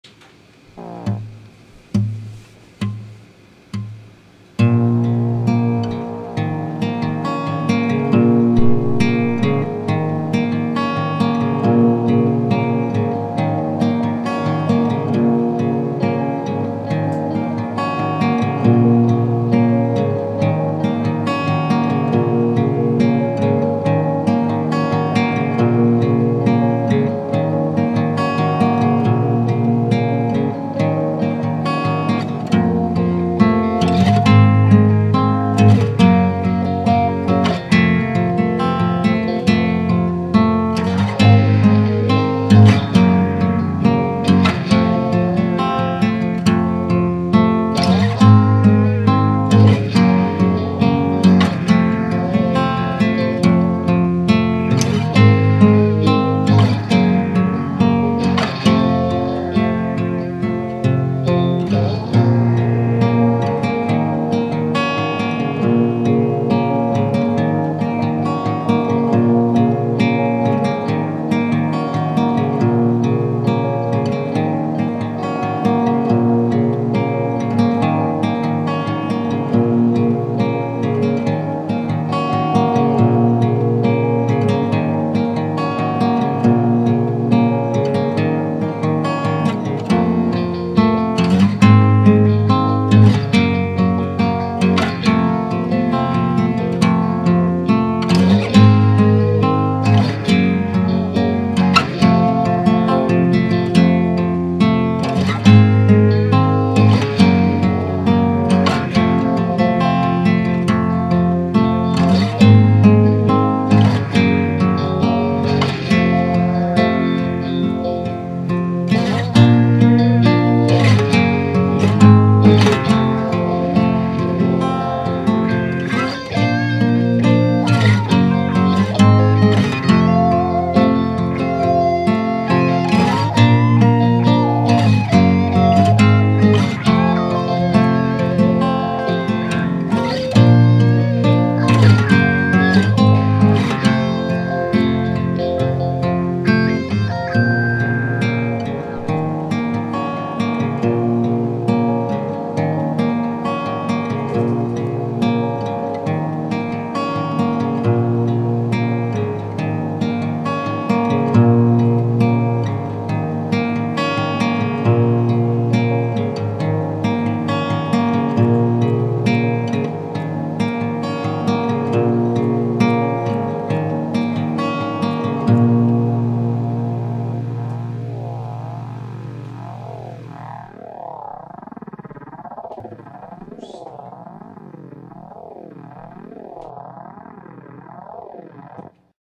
untitled song #2
another old one. brian wrote the song, and i added the electric noise bits